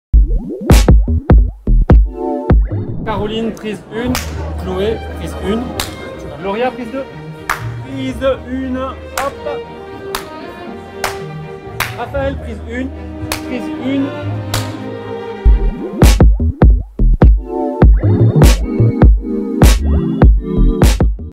Teaser - Capsules vidéos
Rendez-vous la semaine prochaine pour 8 capsules vidéos sur nos membres !

Lors de notre soirée de rentrée, nous les avons interrogé sur leurs métiers, le Club et leurs sources de créativité.
Nous avons hâte de vous présenter ces vidéos !

Réalisation Artigas Films - Laurent Moussinac, motion design Lucas Geraud.